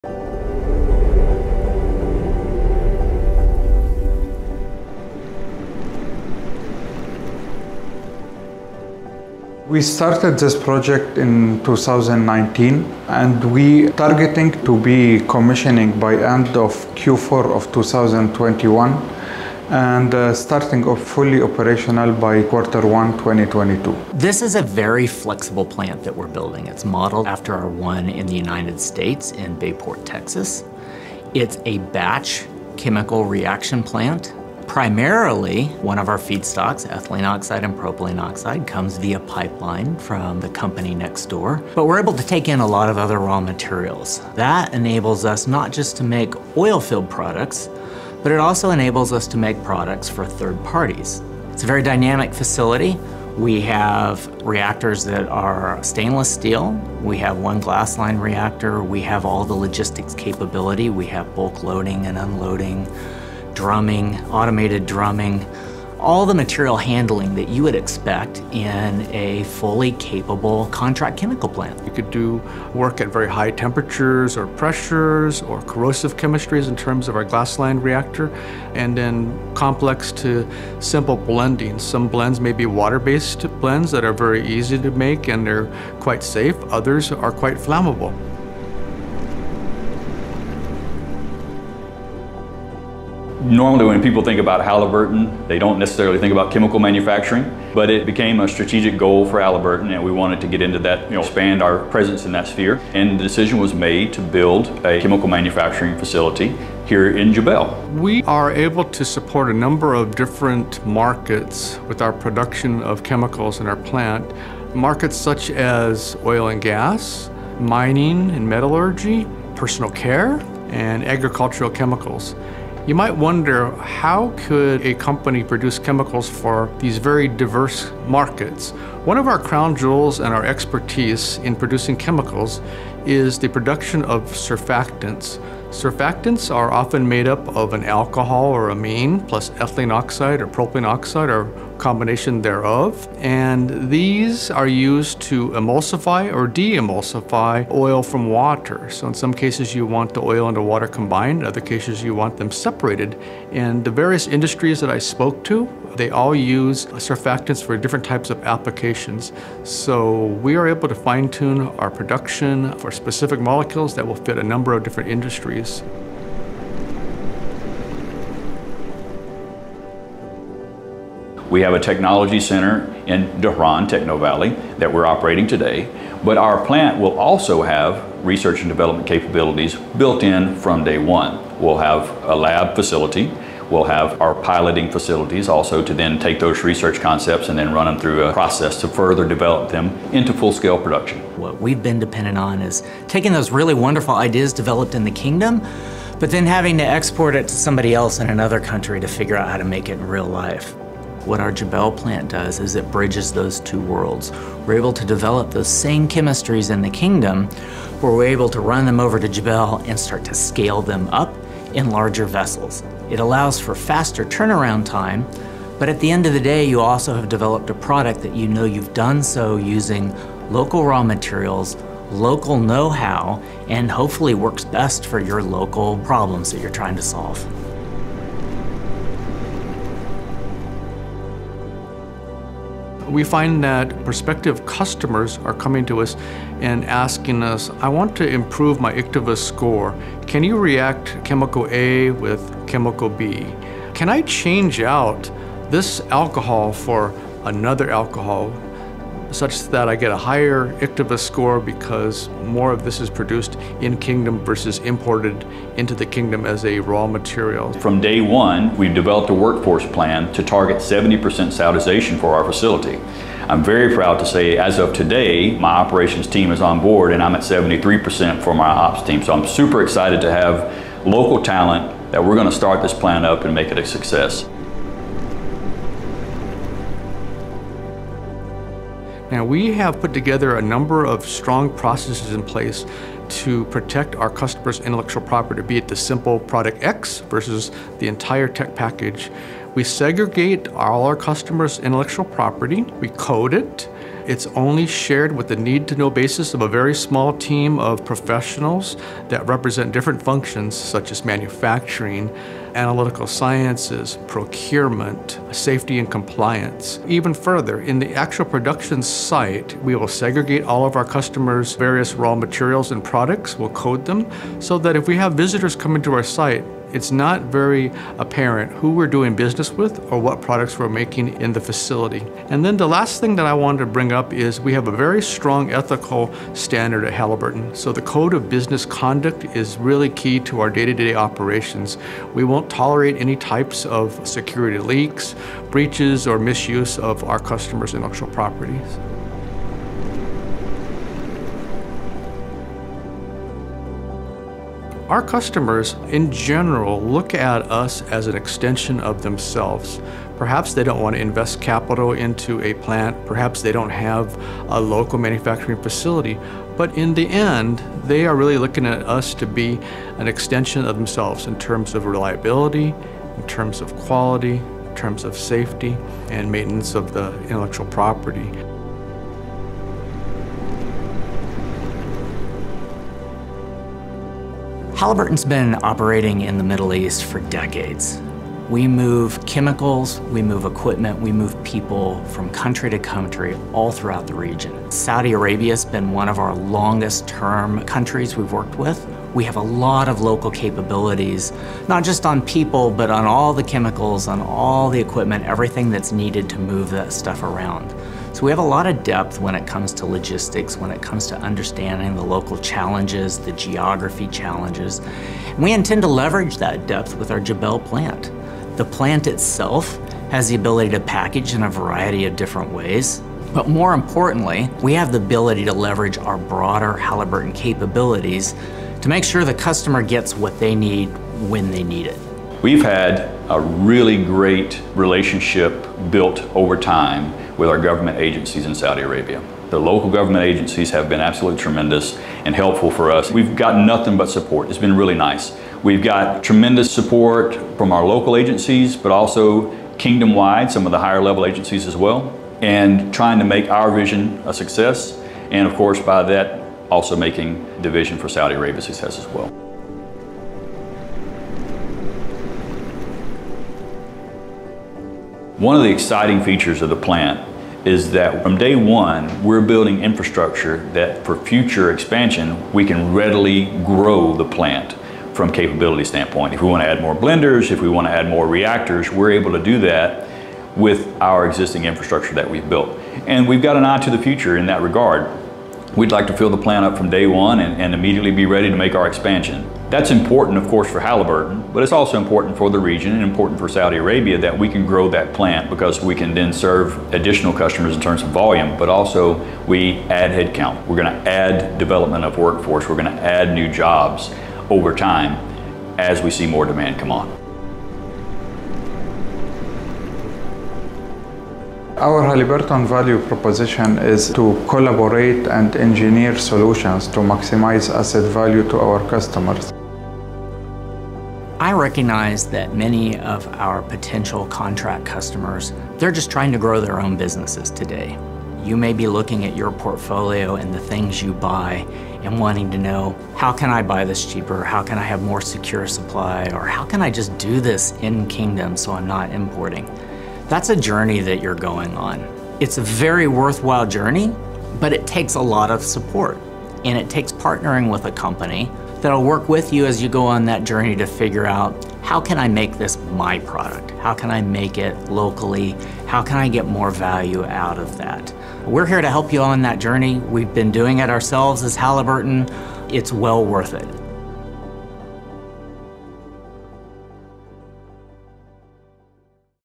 Superior Contract Manufacturing & Tolling Capabilities to Serve the Kingdom of Saudi Arabia
Halliburton’s Saudi Arabia Chemical Reaction Plant is well-positioned to serve multiple markets in the Kingdom beyond the oilfield with flammable blend tolling and simple to complex reactions, including oxide chemistry manufacturing. This world-class facility bridges in-Kingdom chemical research and development with piloting and full-scale commercial production capabilities to strengthen security of supply, shorten lead times and streamline logistics.